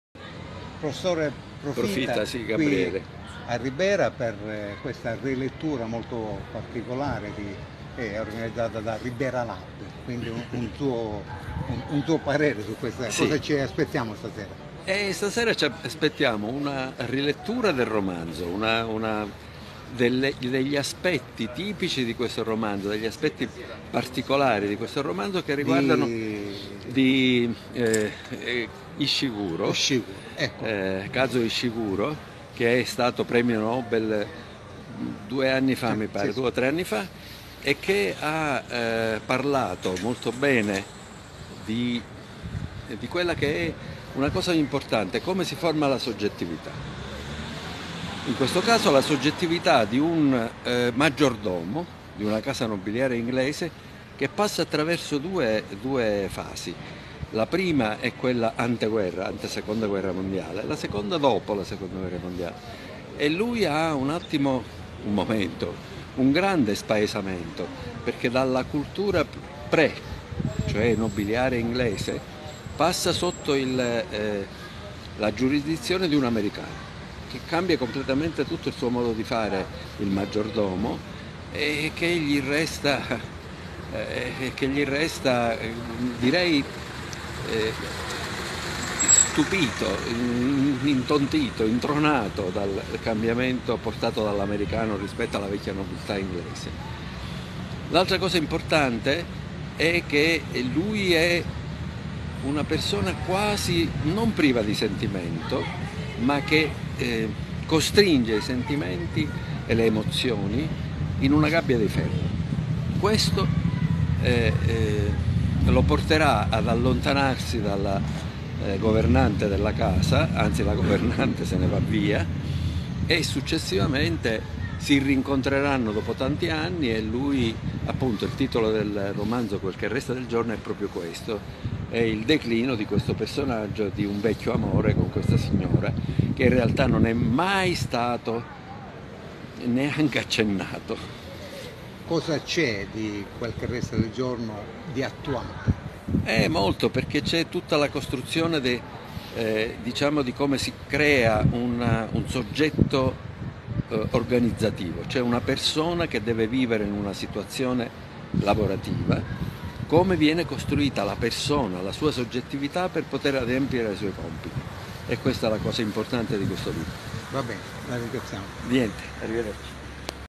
Mimmo Tornambé intervista Gabriele Profitagia'
Mimmo Tornambé intervista Gabriele Profita
gia' Professore ordinario di Psicologia Clinica,
che rilegge, stasera, nella Sala Convegni "QUEL
CHE RESTA DEL GIORNO" di Kazuo Ishiguro.